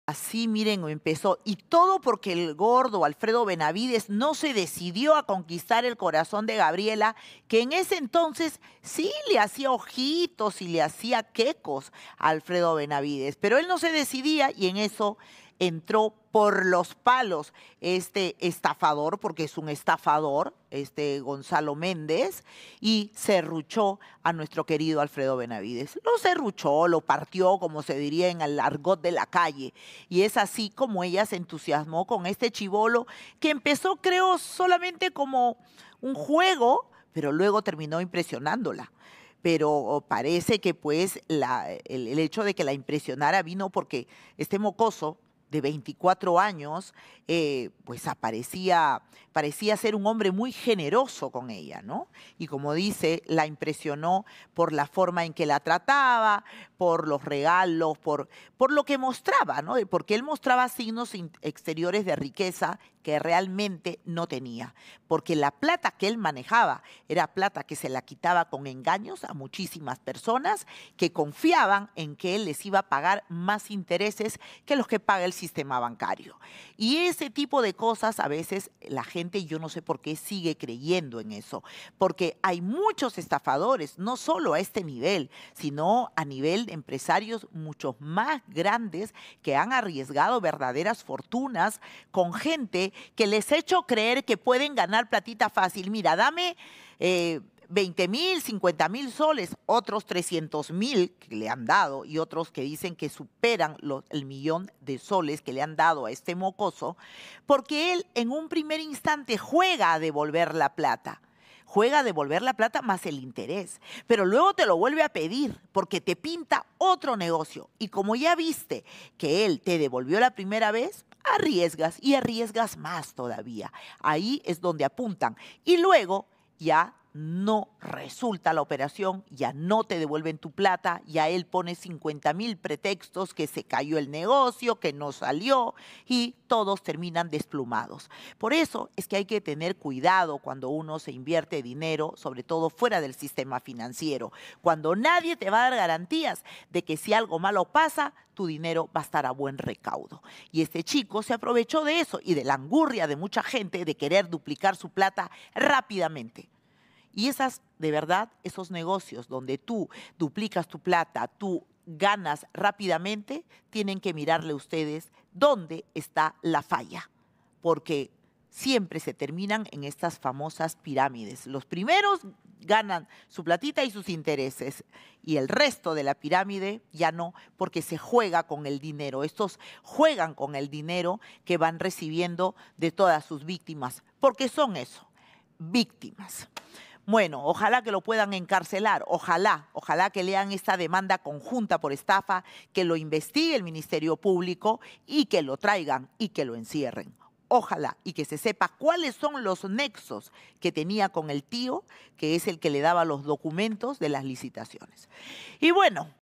Magaly Medina sobre la expareja de Gabriela Serpa: "Ojalá lo puedan encarcelar"
La 'Urraca' espera que las autoridades puedan traerlo cuanto antes al Perú.